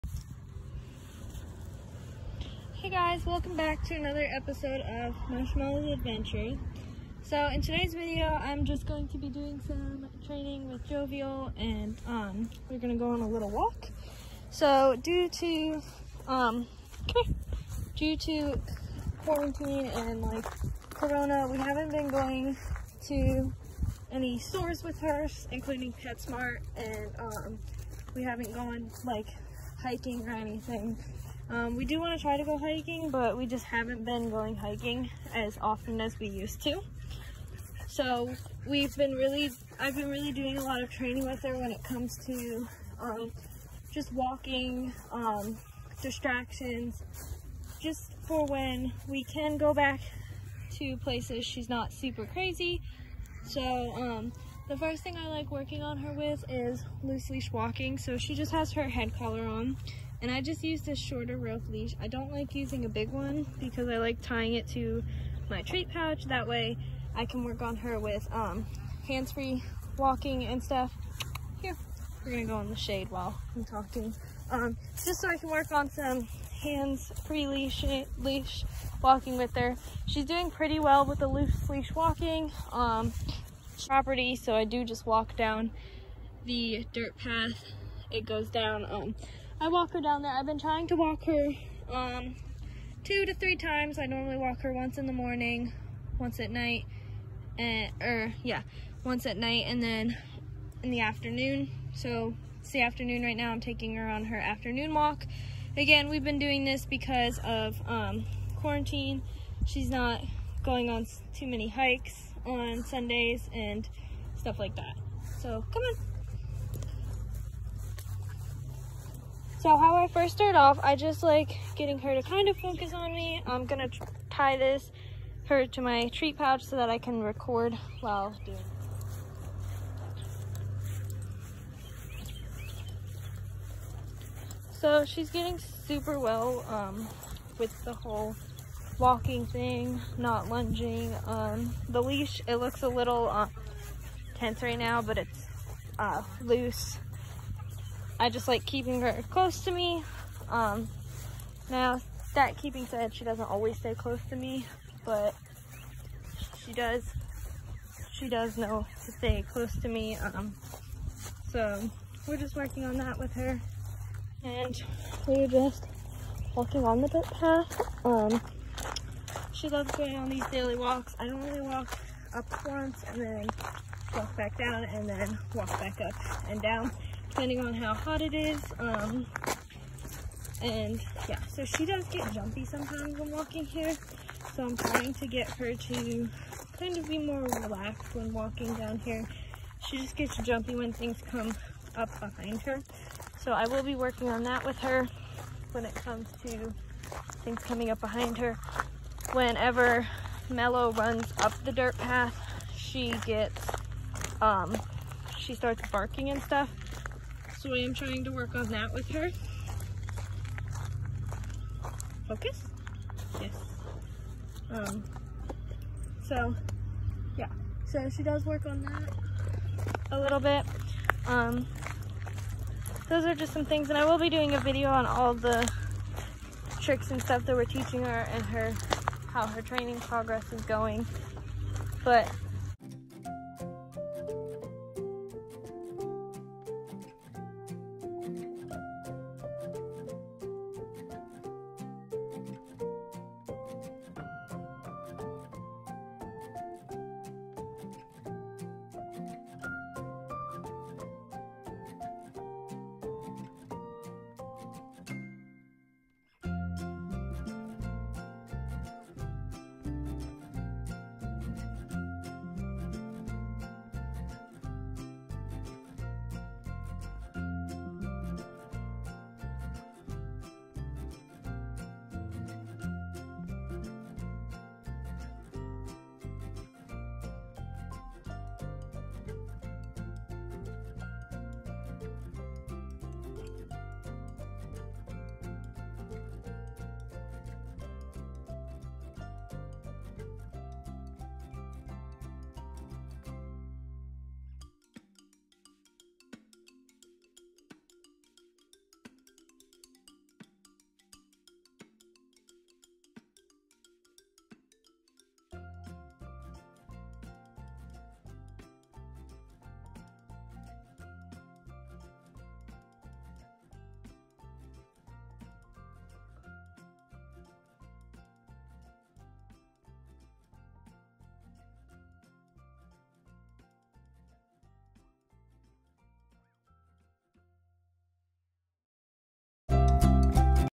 Dog Training vlog
In today’s video me and jovial went on a walk and practiced her lose leash walking.